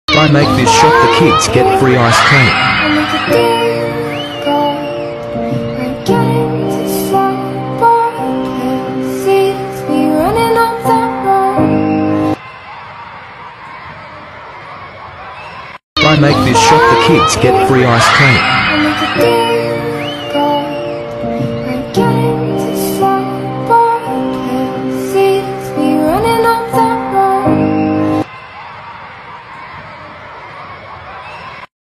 These Kids Got Free Ice Cream After He A Half-Court Shot
Became a superhero with one shot 😂❤️

(via gogetitjohnjohn/IG)